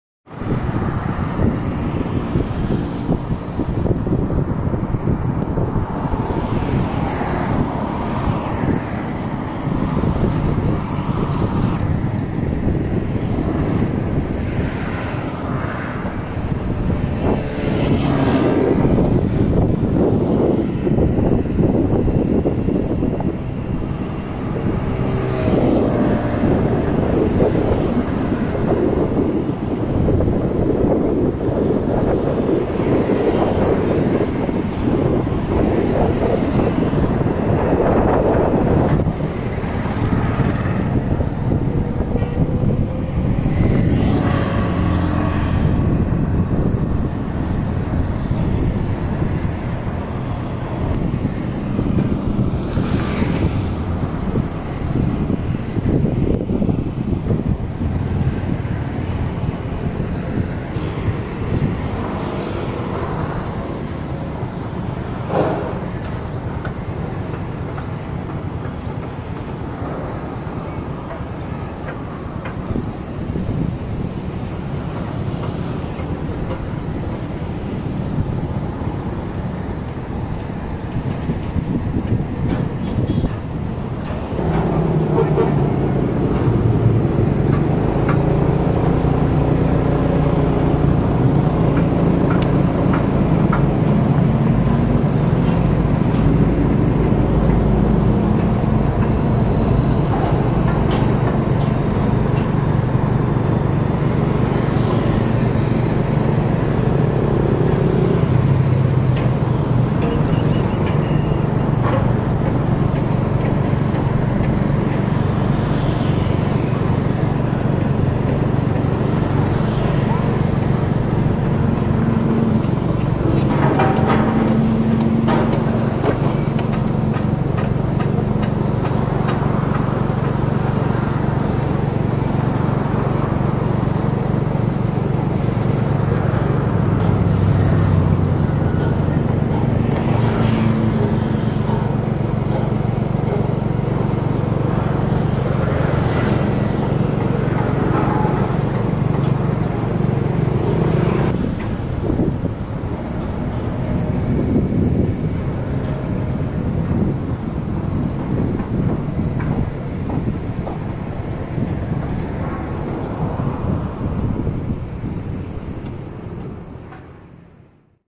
60m road is busy building modern buildings 18-08-2019
Construction modern buildings in Phnom Penh, Cambodia 2019.